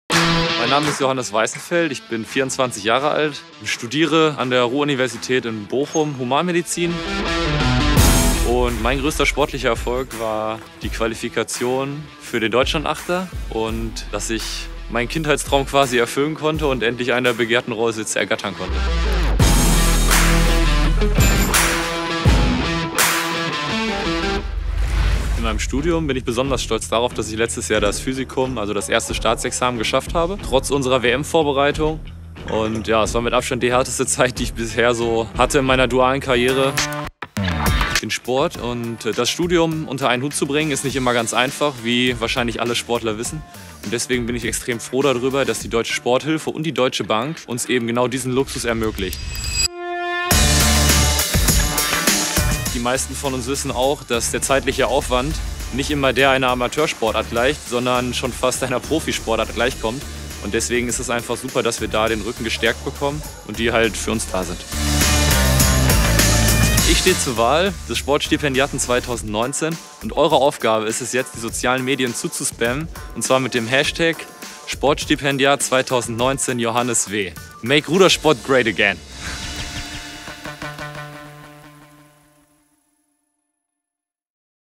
Sport-Stipendiat des Jahres 2019: Johannes Weißenfeld
Warum Rudersportler Johannes Weißenfeld Sport-Stipendiat des Jahres 2019 werden soll? Im Clip erfährst du es! Alle Informationen zu unserer Kooperation mit der Deutschen Sporthilfe gibt es außerdem